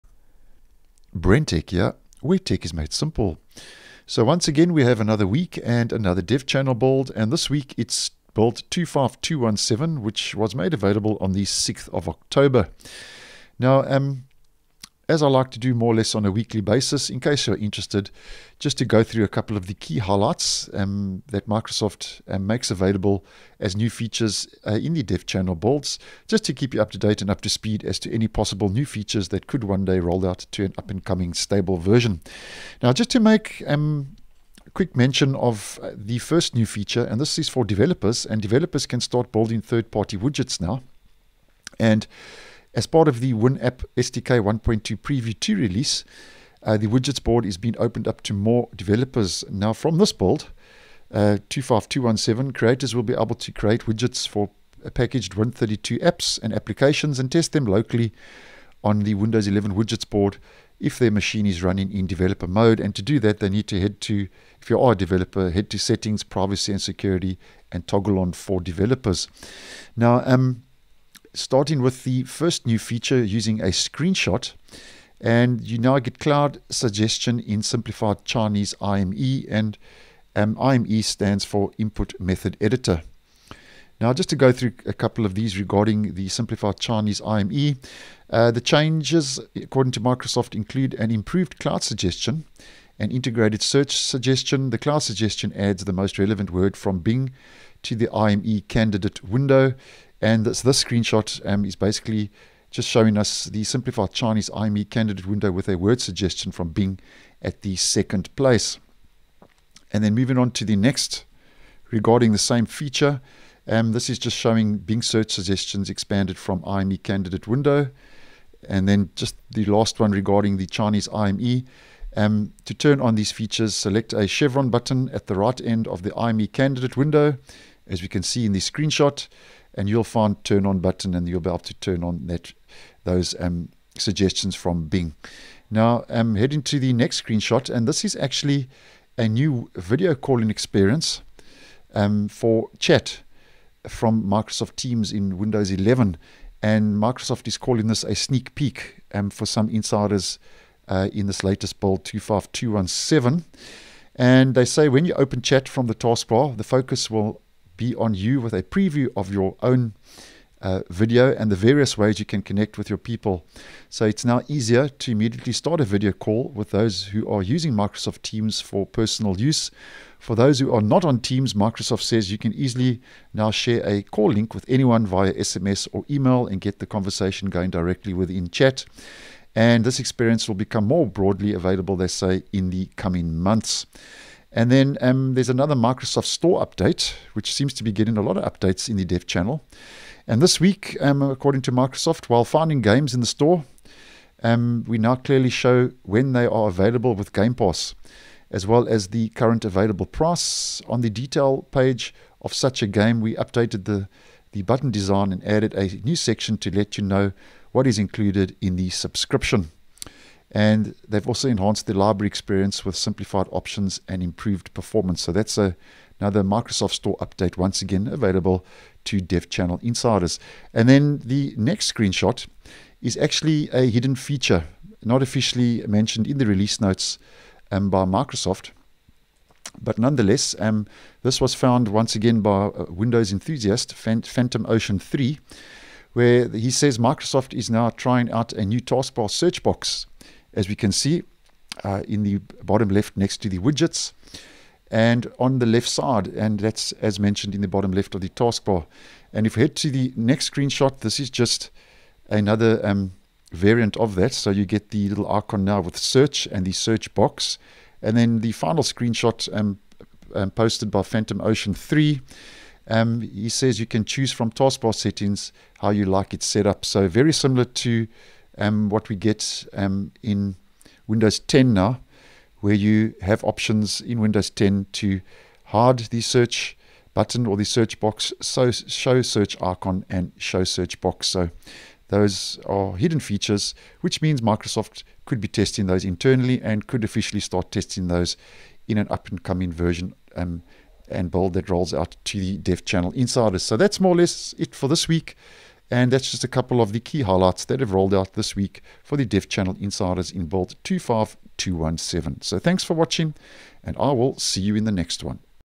Windows 11 Build 25217: New 3rd party Widget support, Teams Chat, Taskbar search and Store update
What's new in this weeks latest Dev Channel insider preview build: Developers can start building 3rd party widgets, Cloud Suggestion in Simplified Chinese IME, a new video calling experience for Chat from Microsoft Teams, a Microsoft Store Update and a new Taskbar Search box on the left with settings (hidden feature).